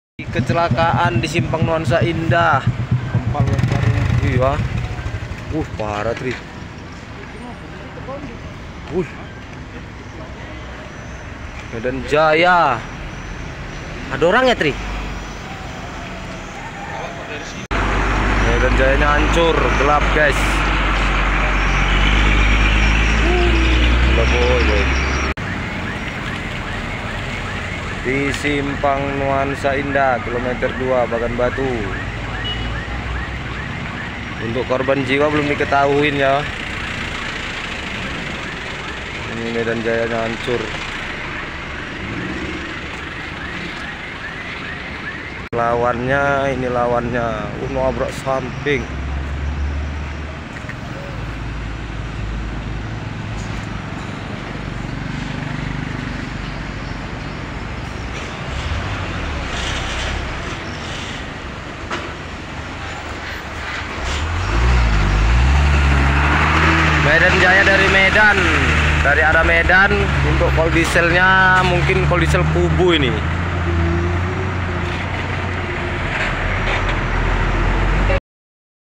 innalilahi ! lakalantas bus Medan jaya vs canter, disimpang nuansa indah kM 2 Bagan batu rohil
bus Medan jaya vs canter